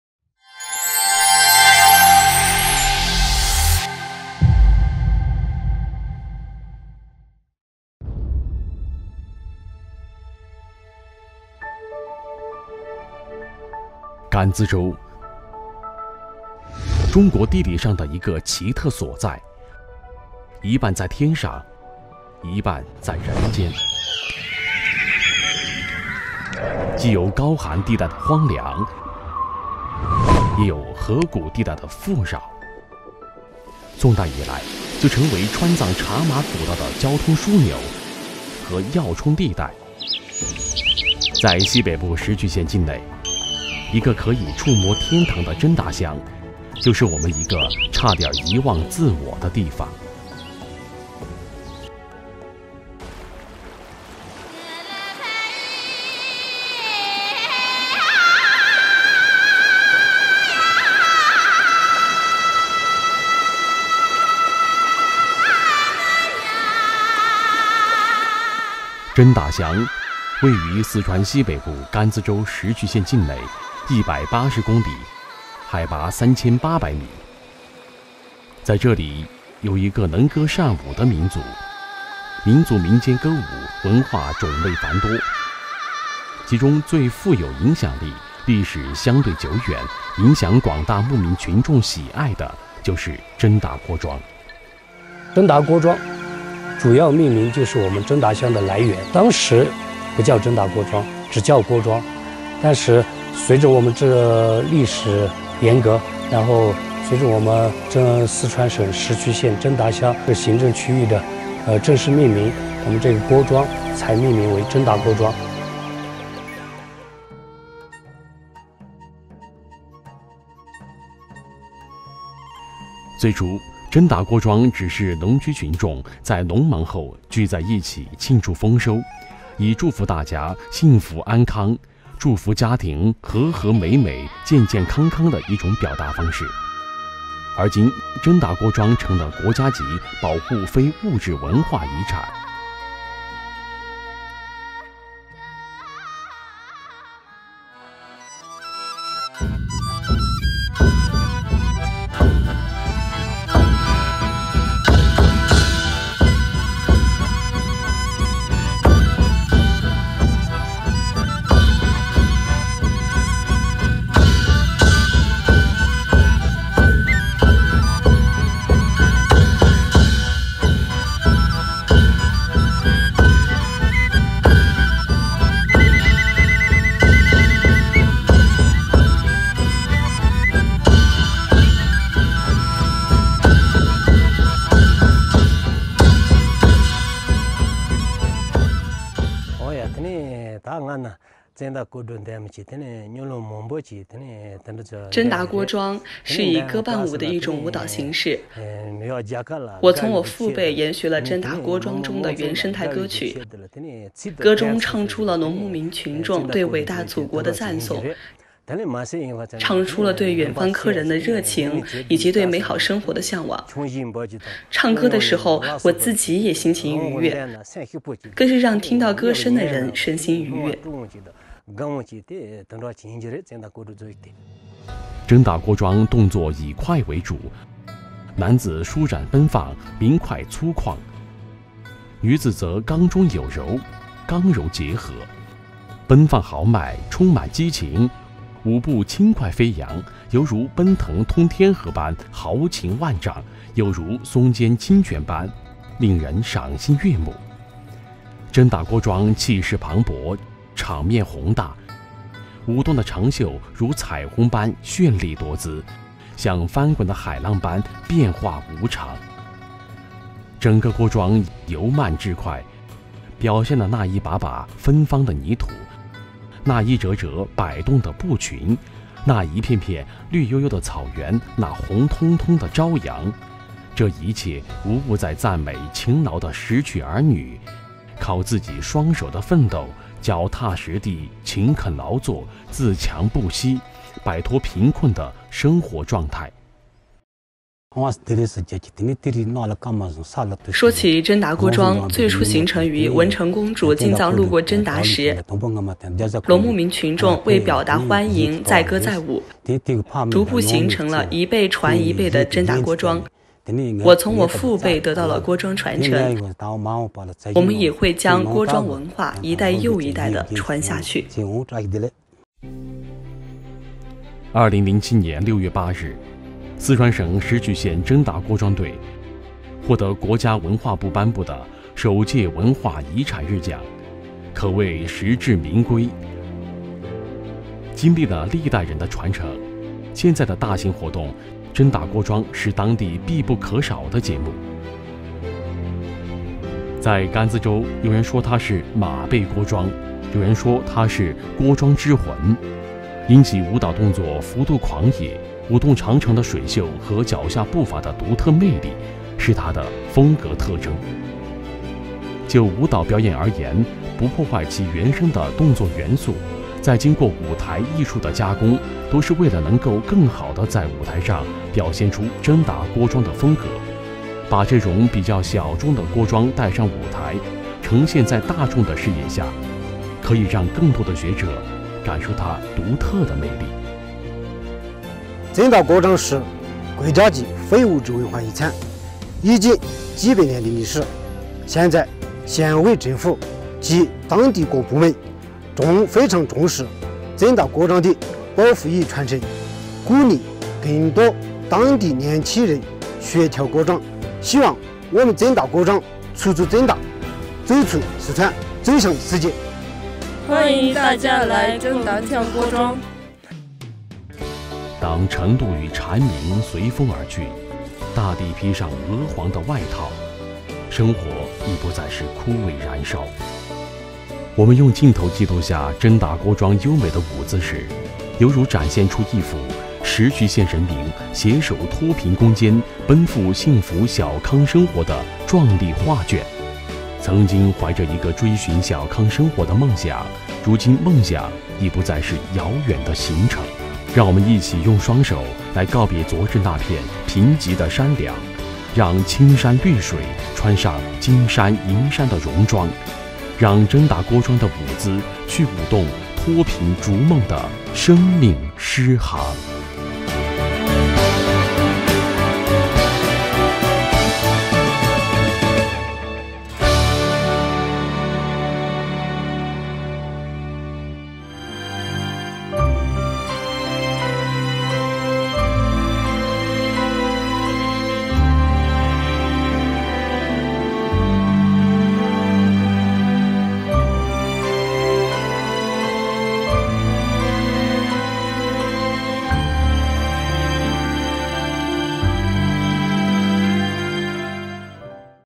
All-round moderate prosperity of Zendaguozhuang
Zhendaguozhuang is an original dance of Zhenda, Ganzi Prefecture. With a long history and a rough and intense performance, it is unique in the Tibetan dance system. Let's follow the documentary into this folk dance.